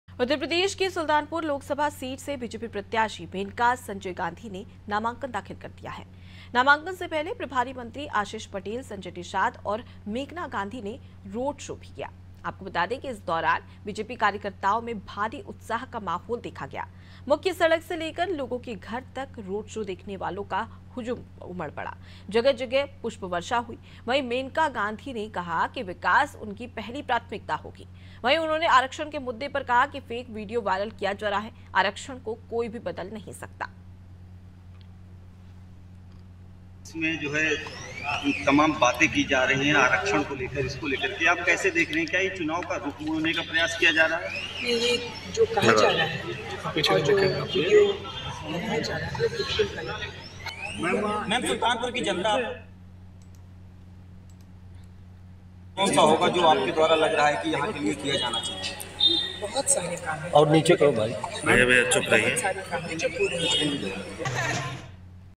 Sultanpur : बीजेपी प्रत्याशी मेनका गांधी भरा पर्चा, मंत्री आशीष पटेल.. संजय निषाद हुए शामिलAnaadi Tv
AIRTEL - 379